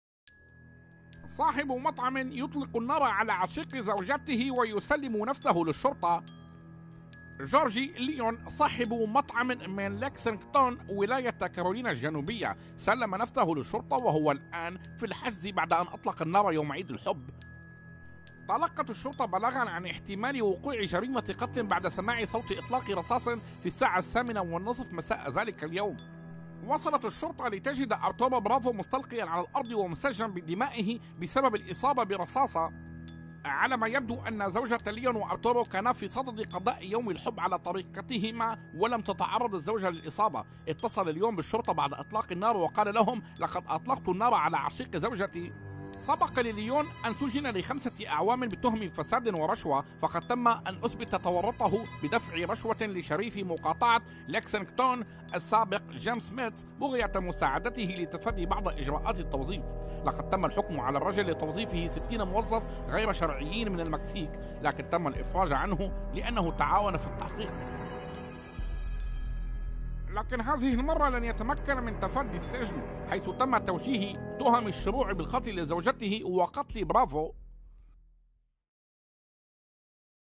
جورجي ليون صاحب مطعم من كارولينا الجنوبية يطلق النار على عشيق زوجته
صاحب مطعم يطلق النار على عشيق زوجته ويسلم نفسه للشرطة
جورجي ليون صاحب مطعمٍ من ليكسنغتون ولاية كارولينا الجنوبية, سلم نفسه للشرطة وهو الآن في الحجز بعد أن أطلق النار يوم عيد الحب.
في يوم عطلة الروانس تلقت شرطة ليكسنغتون بلاغاً عن احتمال وقوع جريمة قتل بعد سماع صوت اطلاق رصاص في الساعة الثامنة والنصف مساء ذلك اليوم.
وصلت الشرطة لتجد أرتورو برافو مستلقياً على الأرض ومسجاً بدمه بسبب الاصابة برصاصة.
على ما يبدو أن زوجة ليون وأرتورو كانا في صدد قضاء يوم الحب على طريقتهما. ولم تتعرض الزوجة للاصابة.
اتصل ليون بالشرطة بعد اطلاق النار وقال لهم: لقد أطلقت النار على عشيق زوجتي.
لم تكن هذه اول مرة يواجه فيها ليون القانون, فقد سبق أن سجن لخمسة أعوام بتهم فساد ورشوة.
فقد تم اثبت تورطه بدفع رشوة لشريف مقاطعة ليكسنغتون السابق جيمس ميتس بغية مساعدته لتفادي بعض اجراءات التزظيف.
لقد تم الحكم على الرجل لتوظيفه 60 موظف غير شرعيين من المكسيك, لكن تم الافراج عنه لأنه تعاون في التحقيق.
لكن هذه المرة لن يتمكن من تفادي السجن, تم توجيه له تهم الشروع بالقتل لزوجته وقتل برافو.